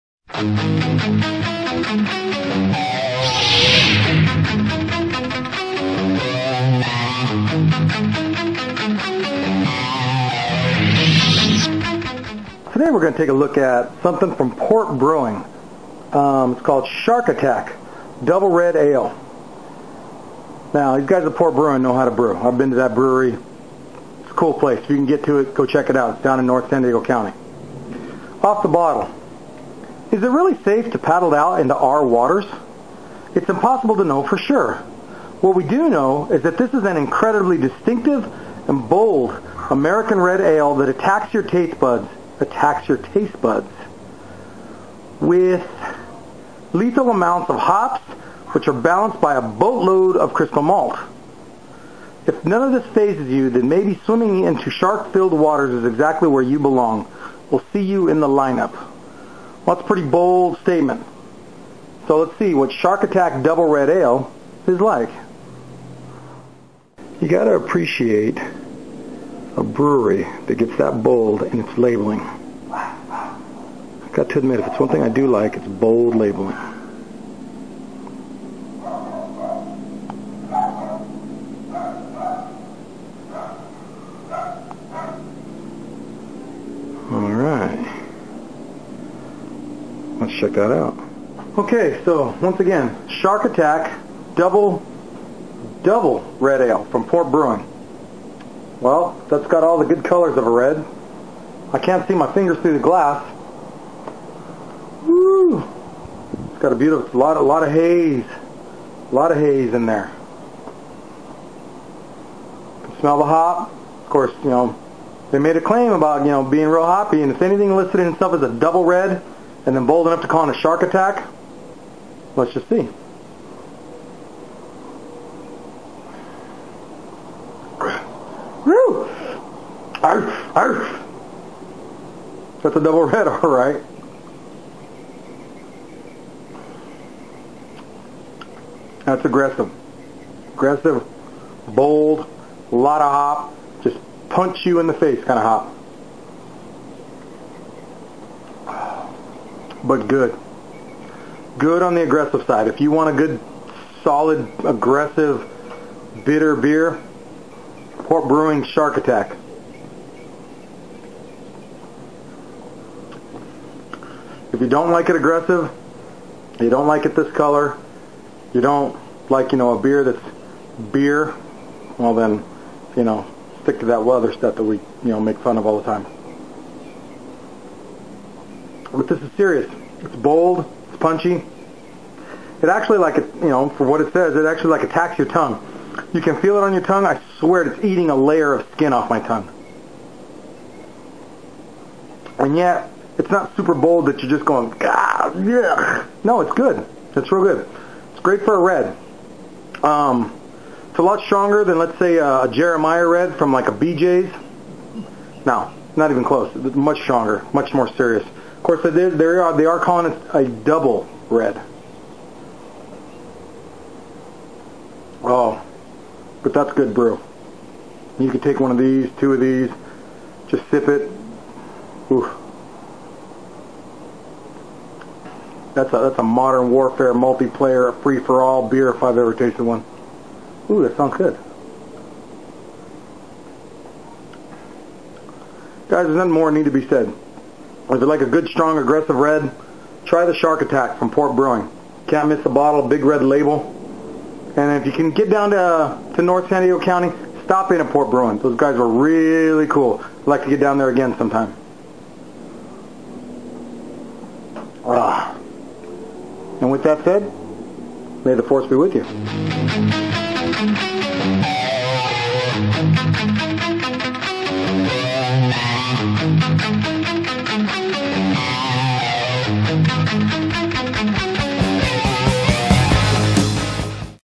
Shark Attack Double Red Ale - Port Brewing.
This Jedi's review of Port Brewing's Shark Attack